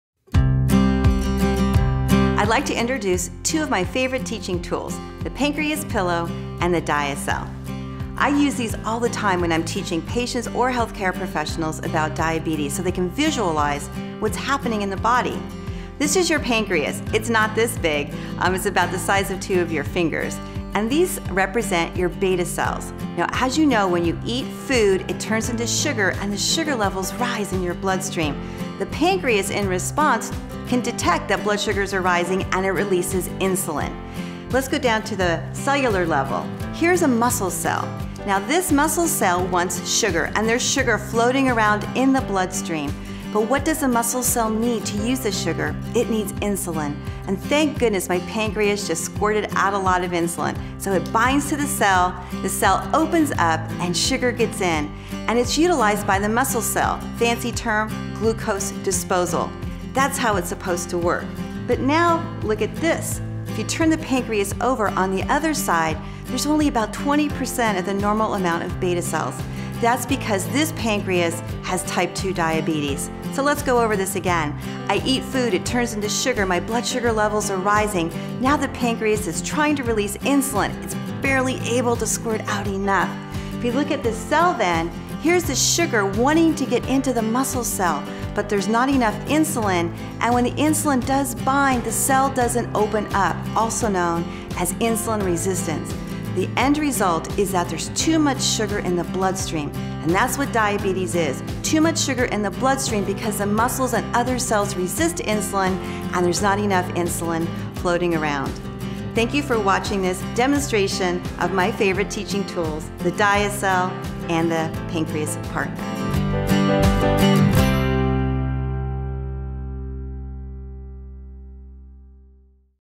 Diacell and Pancreas Partner Demonstration
Beverly Thomassian describes how to use both the Pancreas Partner and DiaCell to describe normal glucose metabolism and how diabetes affects insulin production and action. She also demonstrate strategies to improve glucose control